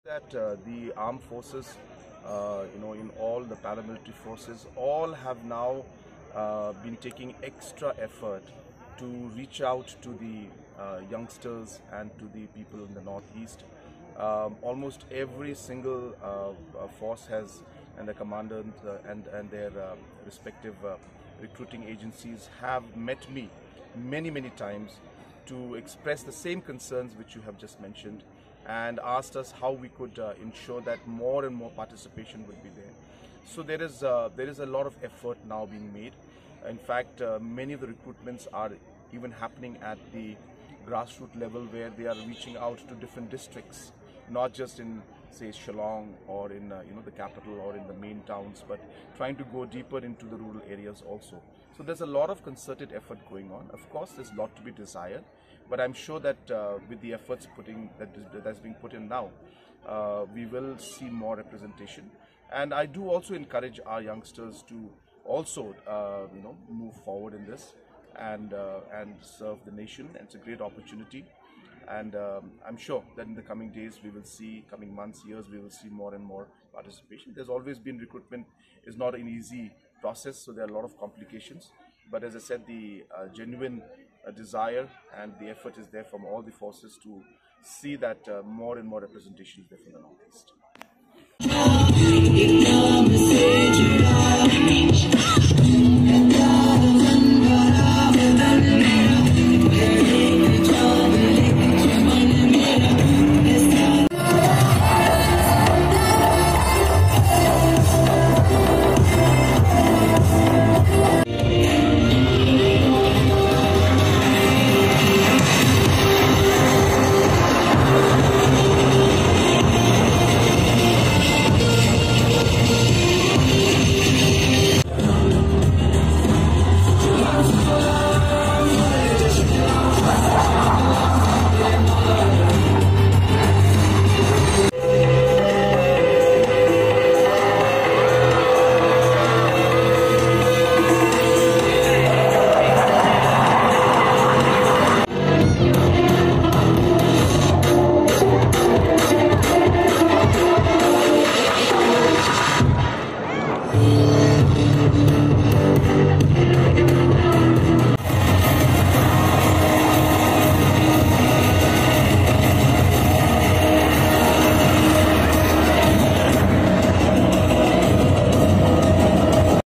INDIAN AIR FORCE’S SURYA KIRAN AEROBATIC TEAM (SKAT) PERFORMS SPECTACULAR AIR SHOW OVER UMIAM LAKE
INDIAN AIR FORCE’S SURYA KIRAN AEROBATIC TEAM (SKAT) PERFORMS SPECTACULAR AIR SHOW OVER UMIAM LAKE SHILLONG